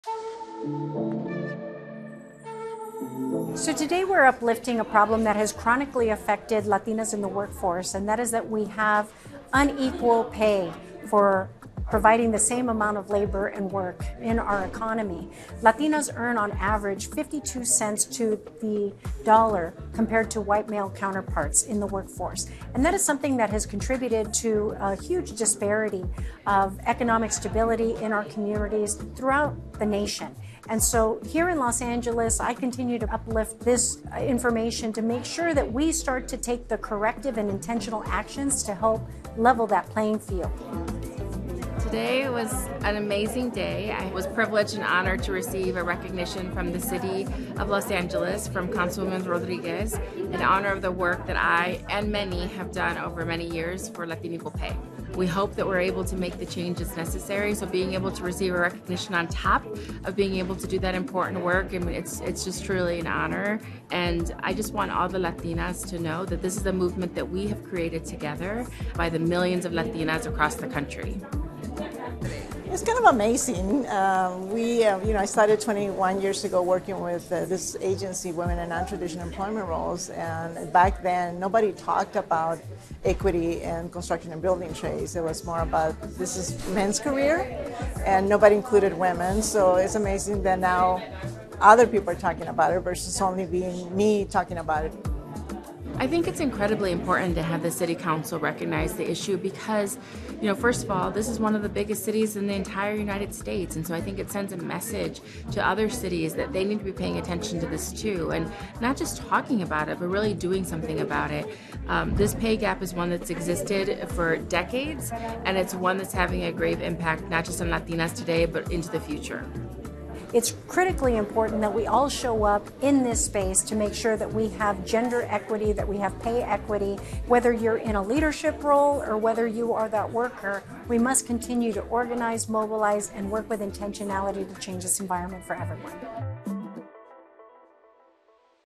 Latina Equal Pay Day
Councilwoman Monica Rodriguez recognized Latina Equal Pay Day at City Hall by honoring two women fighting for women's representation and equity in the workforce.